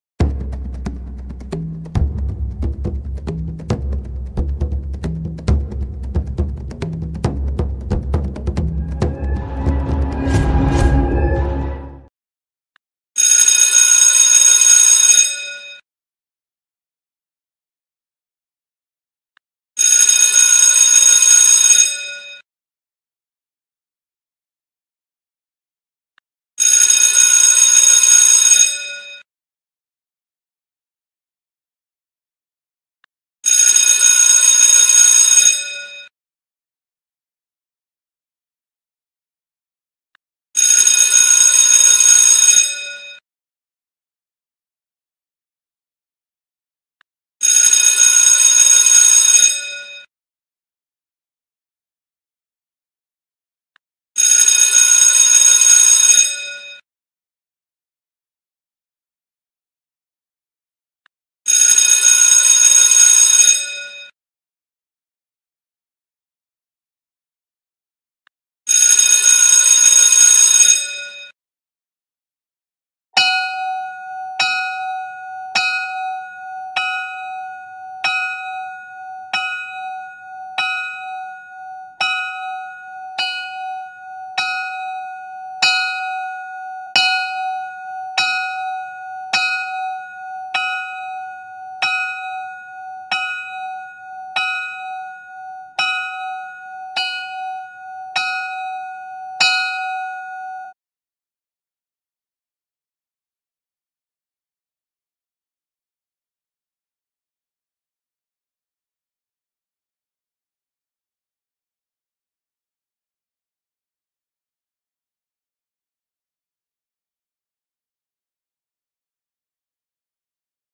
School Bell Sound Effects | No Copyright
This video features a variety of school bell sound effects that are free to use for any purpose. The sounds are clear and high quality, and they can be used to create a variety of different projects. Whether you're making a video for school, work, or just for fun, these sound effects can help you add a touch of realism and excitement.

0:00 Channel Intro
0:12 School Bell Sound Effect
1:12 Old School Bell Sound Effect
1:40 End Screen